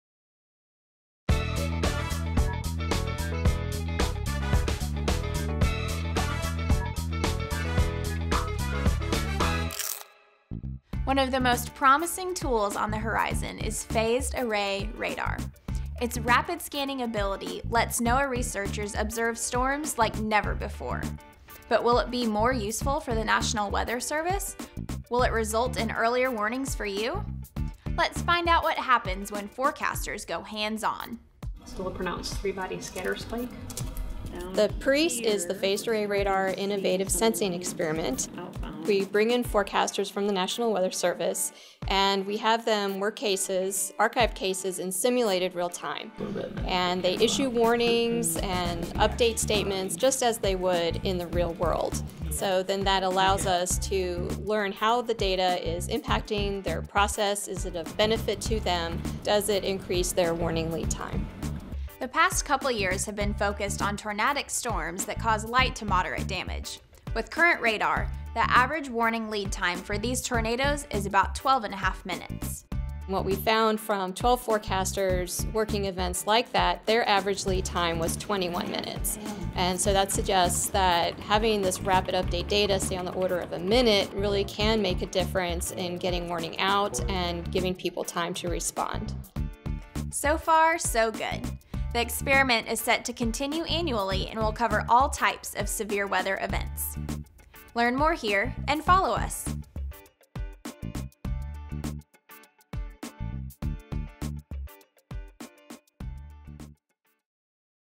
Bite-Sized Science: Hands-On Phased Array Radar
PARISE is the Phased Array Radar Innovative Sensing Experiment.  The NOAA National Severe Storms Lab bring in forecasters from the NWS to have them work archived cases simulated in real-time.  The experiment helps researchers determine what benefits - like extended warning lead-time - phased array radar could provide forecasters.